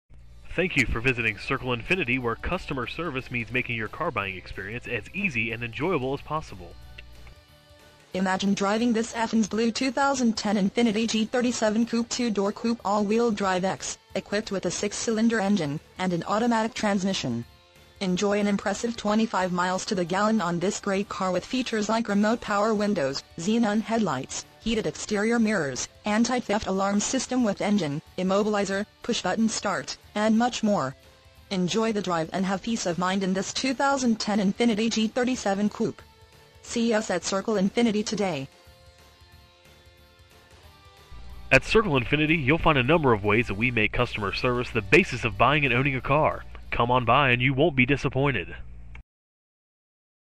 2010 Infiniti G37 Coupe Circle Infiniti West Long Branch, NJ 07764
Infiniti G Coupe Check out this Athens Blue Infiniti G37 Coupe 2 Dr Coupe AWD, equipped with a 6 Cylinder engine and an automatic transmission. Enjoy an impressive 25 miles to the gallon on this great car with features like Remote Operation Power Windows, Xenon Headlights, Heated Exterior Mirrors, Anti-Theft Alarm System With Engine Immobilizer, Push-Button Start, Rear View Camera, Electronic Brakeforce Distribution, Power Door Locks With Anti-Lockout Feature, Speed Sensitive Front Wipers, Front Fog Lights, Speed Sensitive Central Locking Power Door Locks, Emergency Braking Assist, Rear View Monitor, Speed-Proportional Power Steering, Power Driver and Passenger Seating, 6 Disc Cd Player In Dash, Heated Driver and Passenger Seating, Front Air Conditioning With Automatic Climate Control, Xm Satellite Radio, and much more. Enjoy the drive and have peace of mind in this Infiniti G37 Coupe. See us at Circle Infiniti today!
MILES: 47,655
VIN:JN1CV6EL8AM153081

Circle Infiniti
301 Highway 36
West Long Branch, New Jersey 07764